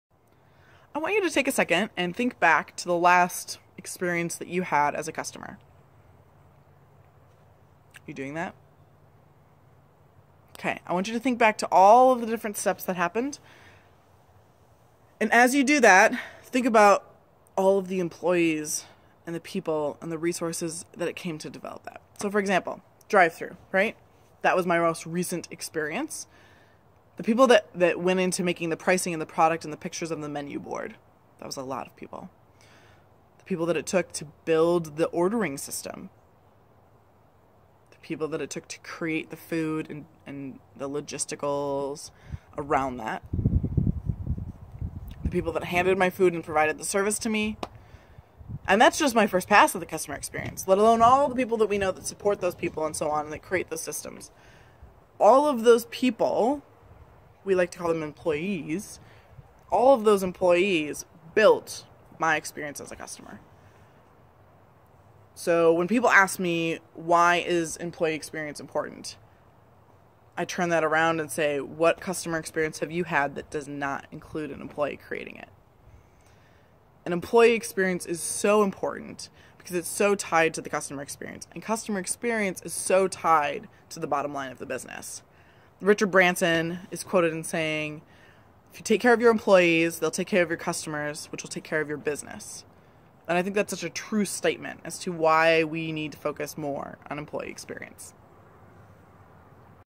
Anna Haug - Pre-CFN18 Video2
Anna Haug is an experience designer and startup advisor at companies including YMCA and Western National. Anna is a strong customer experience community advocate and is one of the Twin Cities CXPA leaders.